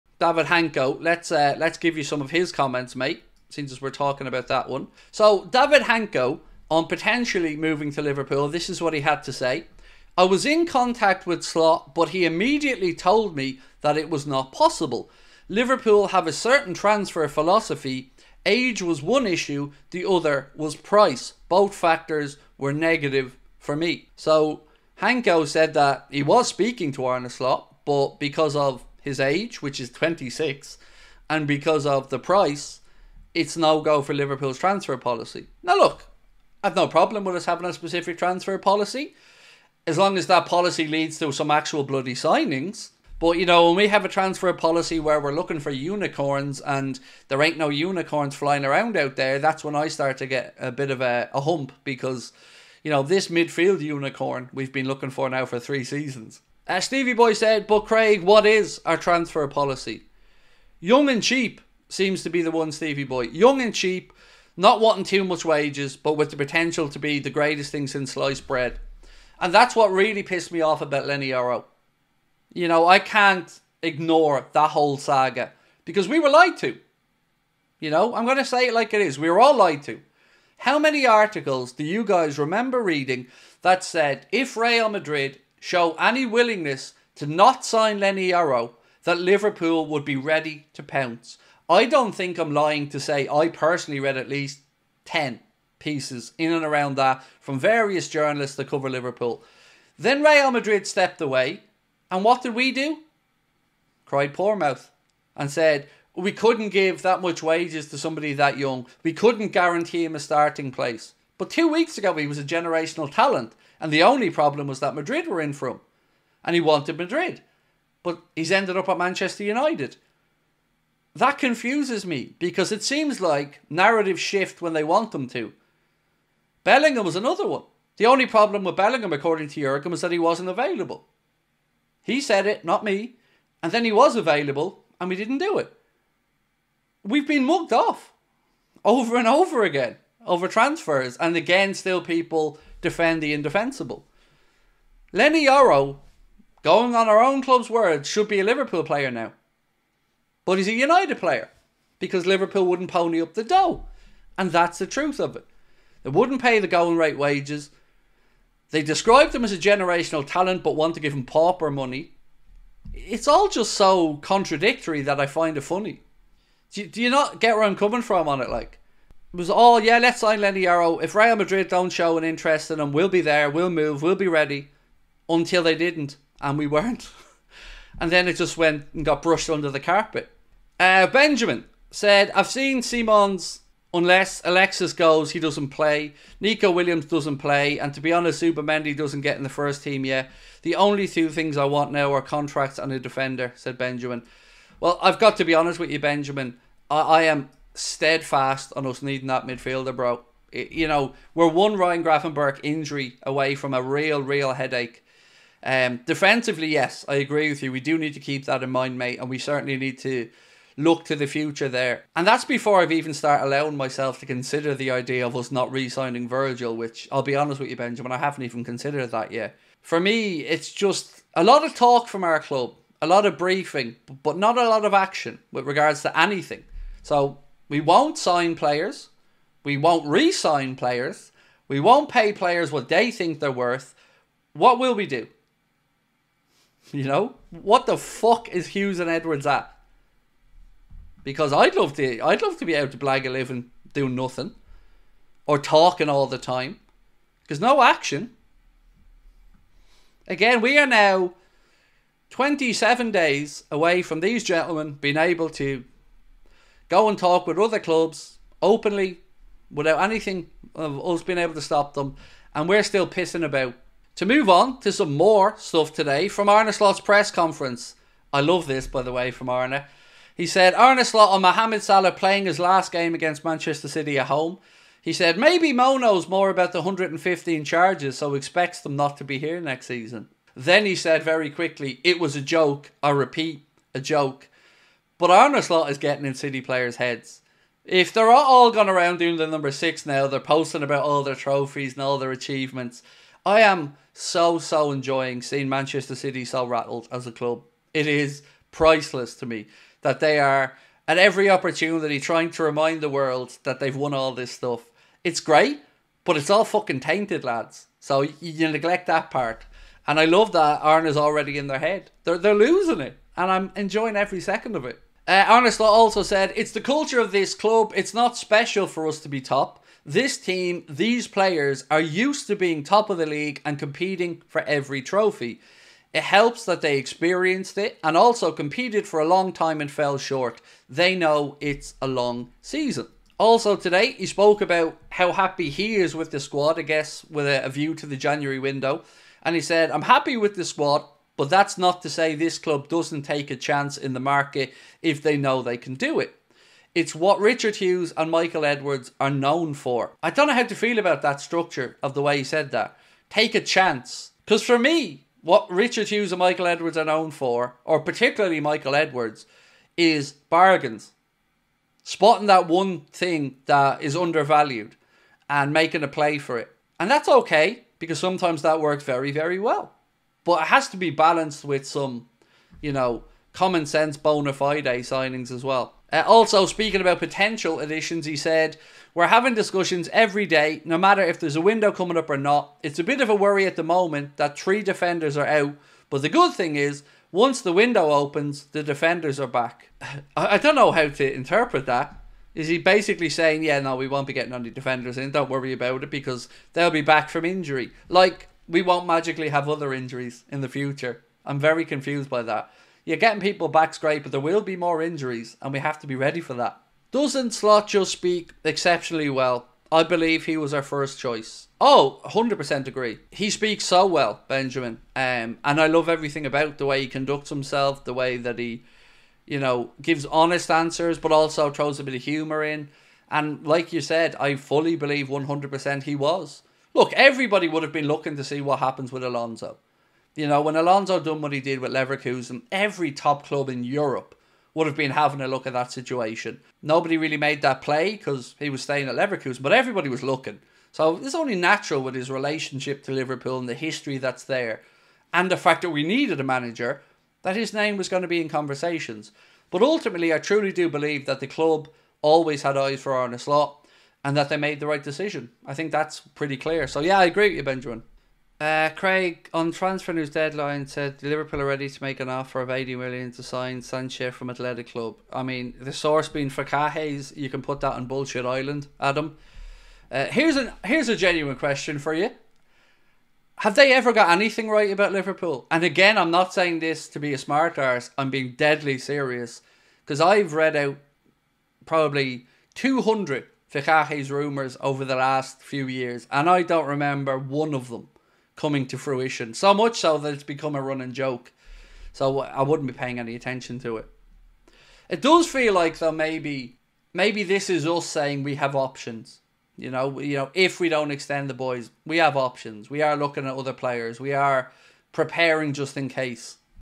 LIVERPOOL TRANSFER STRATEGY REVEALED
Craig Houlden discusses the latest Liverpool news, transfer rumours, injury updates and more here on Anfield Agenda clips